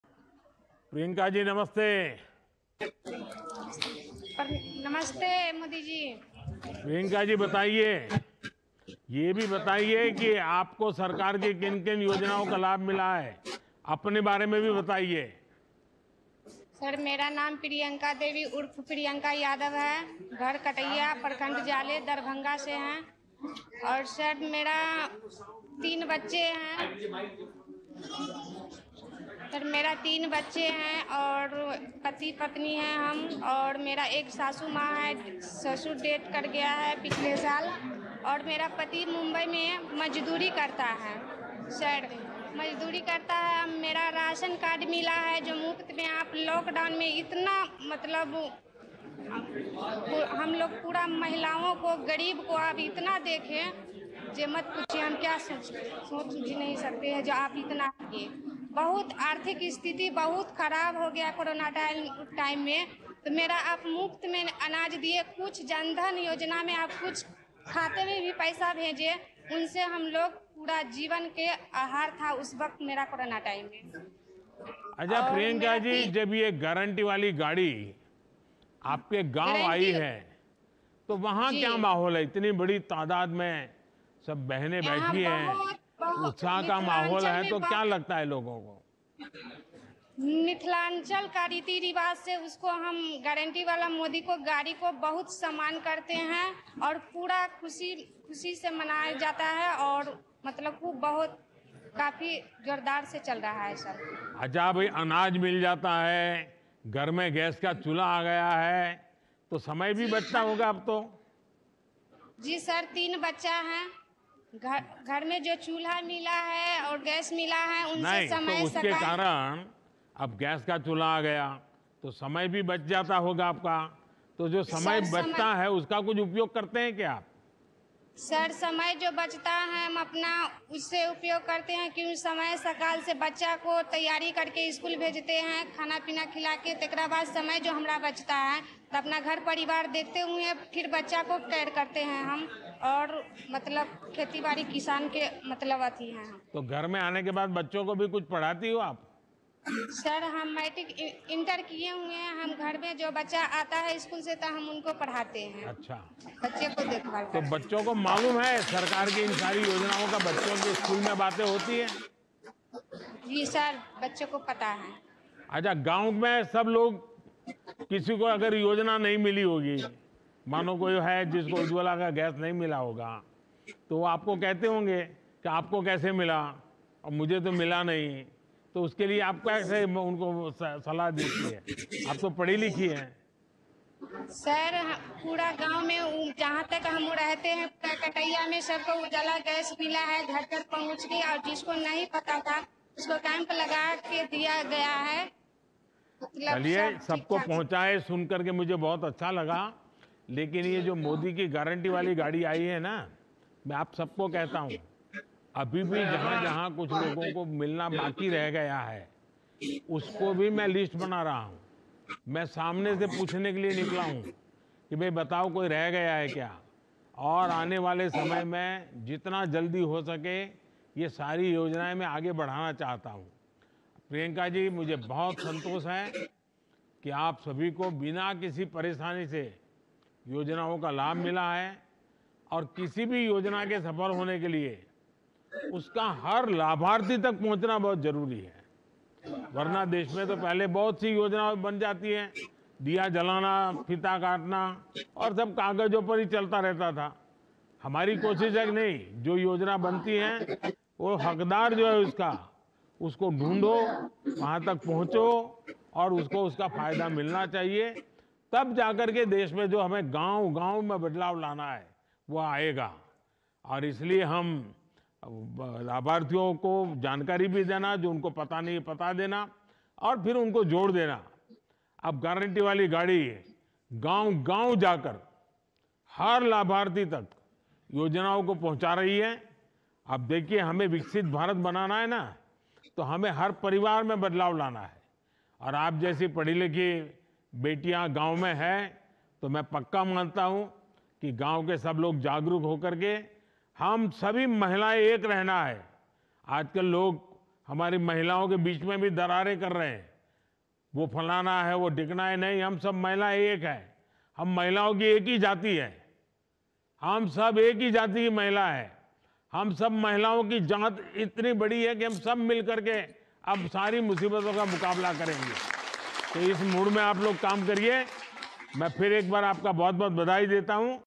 Priyanka Devi reminisces how Modi Government empowered Poor
In an interaction with Prime Minister Narendra Modi, Priyanka Devi reminisced how Modi's guarantee has enabled the empowerment of poor, especially during the COVID pandemic. She said how Ujjwala Yojana has enabled her and many women alike to be able to save their cooking time and send their children to schools also focusing on their education.